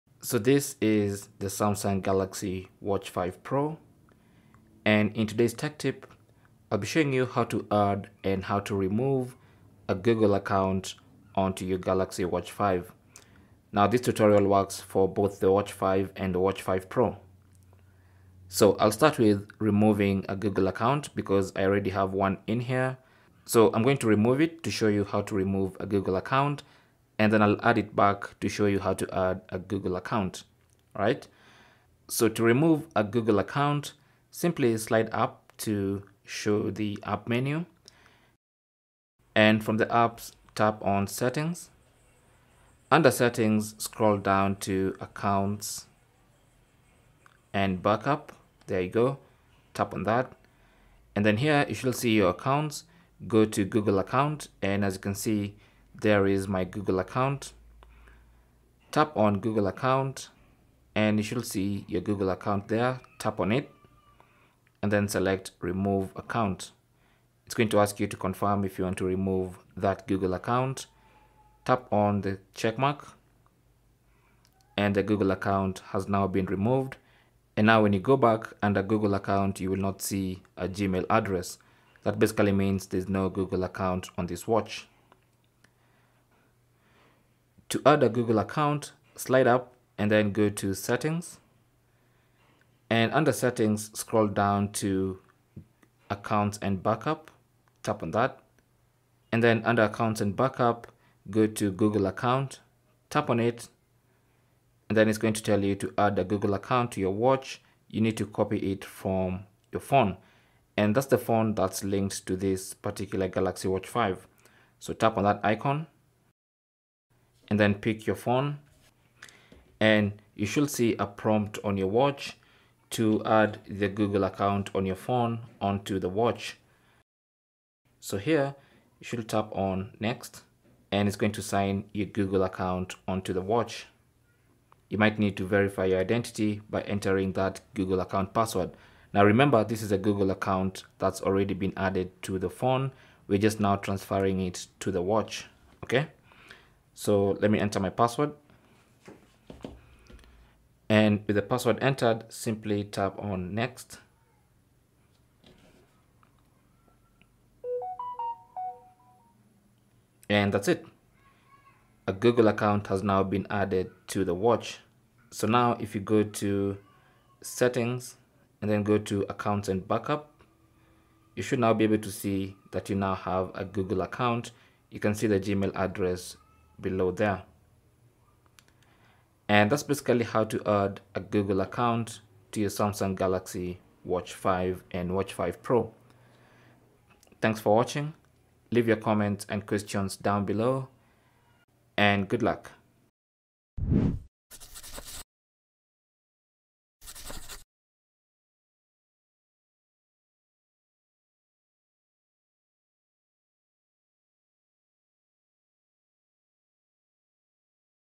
Samsung Galaxy Watch 5 / 5 Pro: How To Add or Remove a Google Account
Learn how to add or remove a Google account on your Samsung Galaxy Watch 5 and Watch 5 pro.

In this video:
0:00 - Intro
0:17 - How To Remove Google Account From Galaxy Watch 5
1:27 - How To Add Google Account To Galaxy Watch 5
3:00 - Outro

Samsung Galaxy Watches (Amazon US)

Cell Phone Tripod Adapter